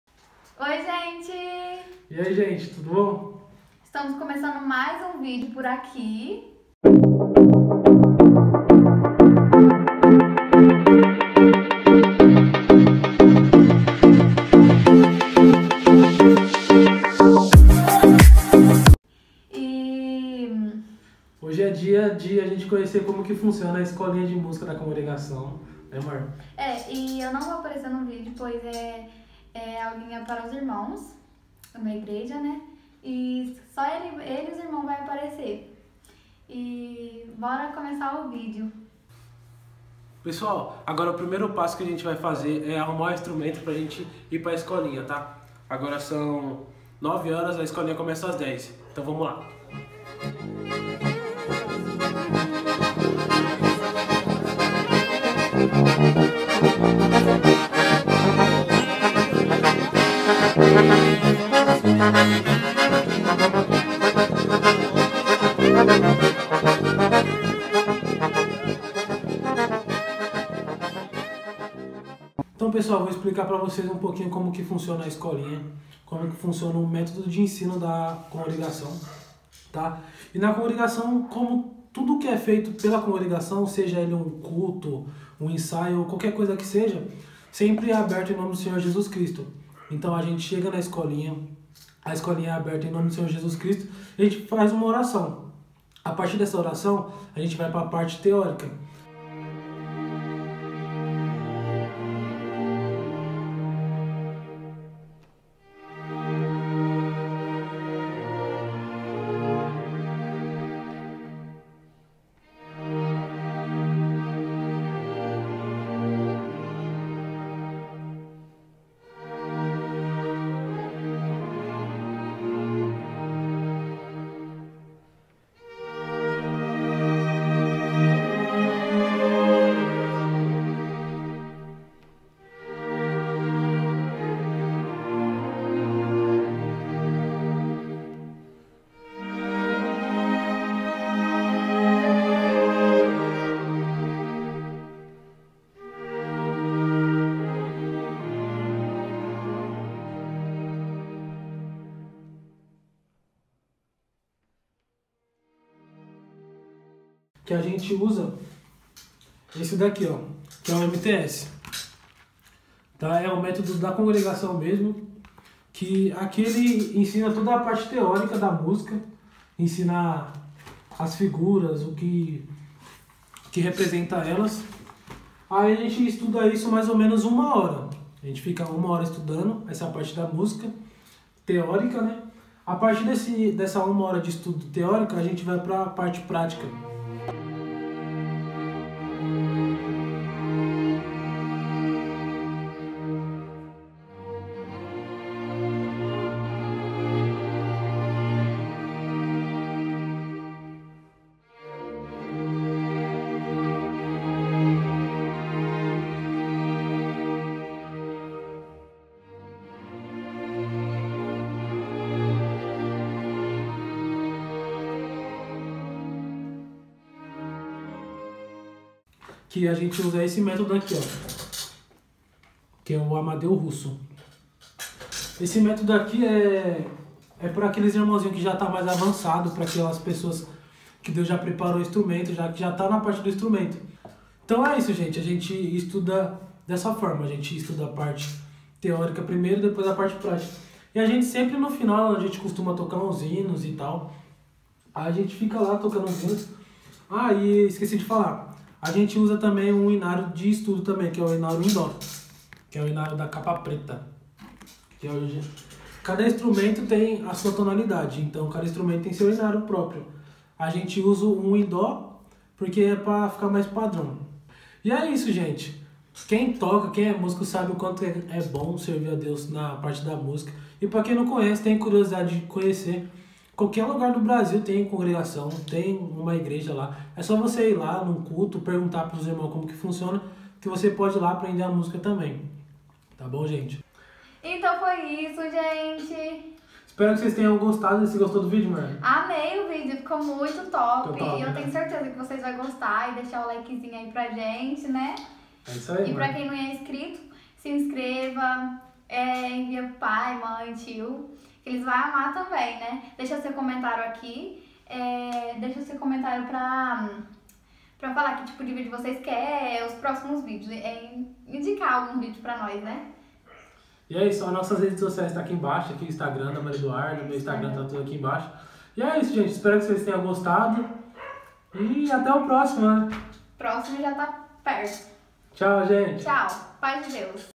Um dia na escolinha de música da congregação cristã no Brasil !!!
Veja como funciona a escola de música da congregação cristã no Brasil ,a maior orquestra do MUNDO !

Esperamos muito que tenham gostado do vídeo !!!